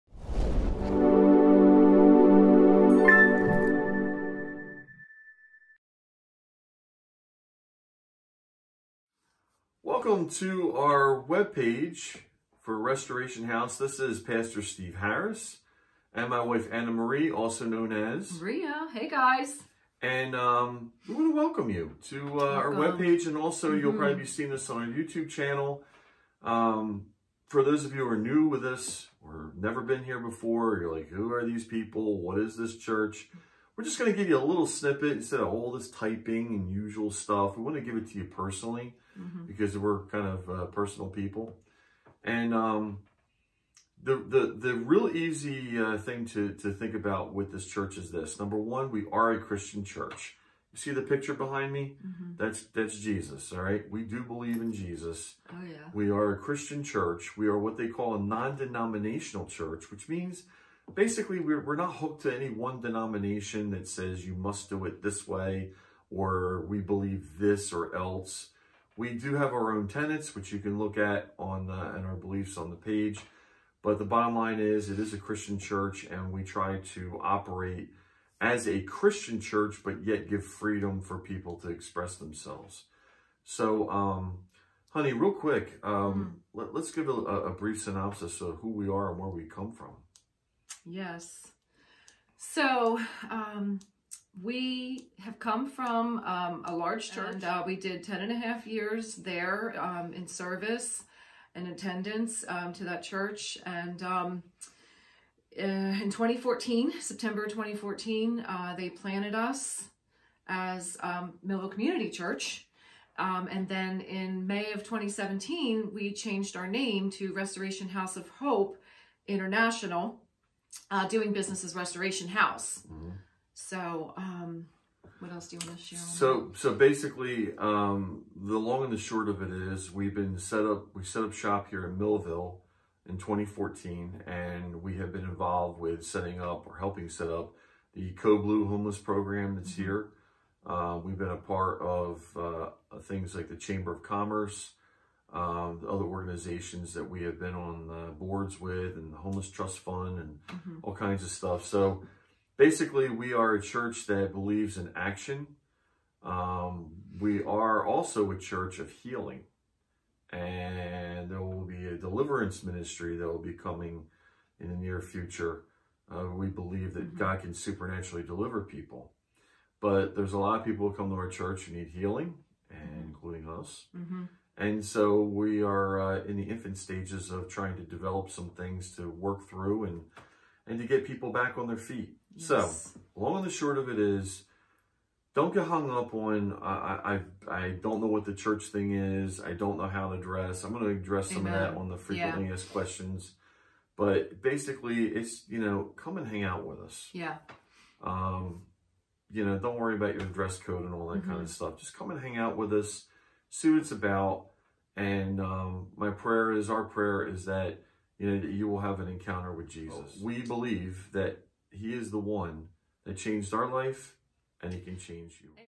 Introduction to the Restoration House Web page
A brief bio of our church with Pastor Steve and Ria Harris